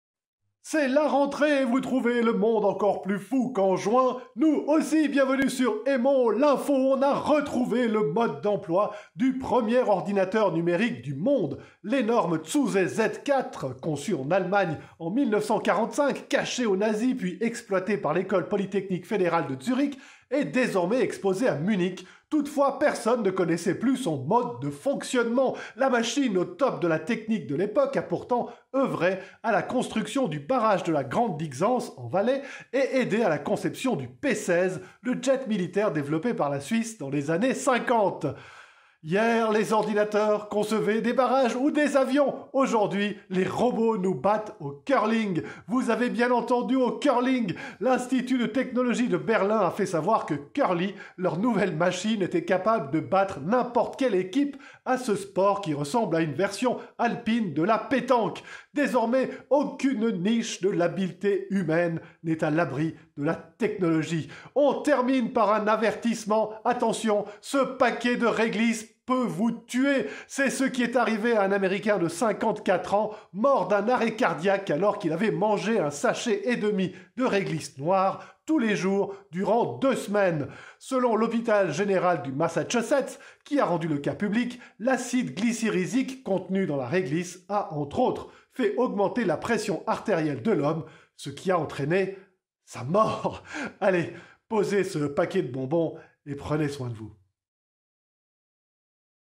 Vendredi 25 septembre
De quoi briller devant la machine à café (et apprendre que la réglisse peut vous tuer).